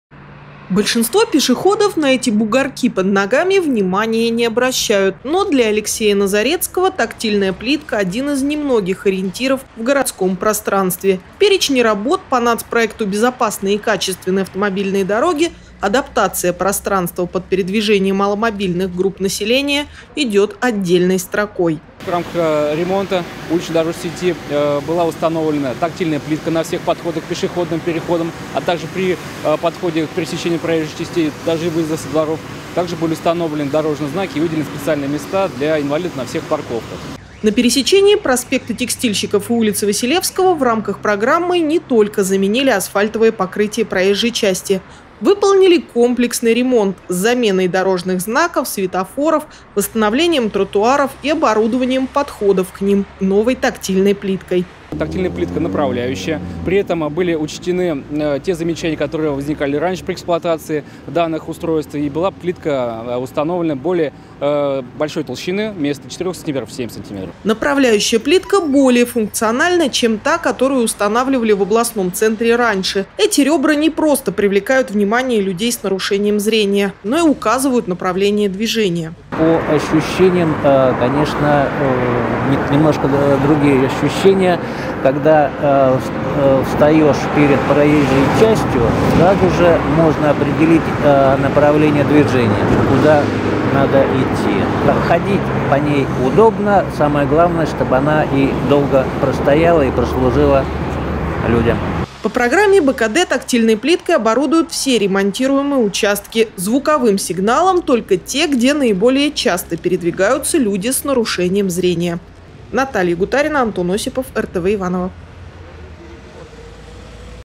Ориентир под ногами
Большинство пешеходов на эти бугорки под ногами внимания не обращают, но для Алексея Назарецкого тактильная плитка - один из немногих ориентиров в городском пространстве. В перечне работ по нацпроекту  «Безопасные и качественные автомобильные дороги» адаптация пространства под передвижение маломобильных групп населения идет отдельной строкой.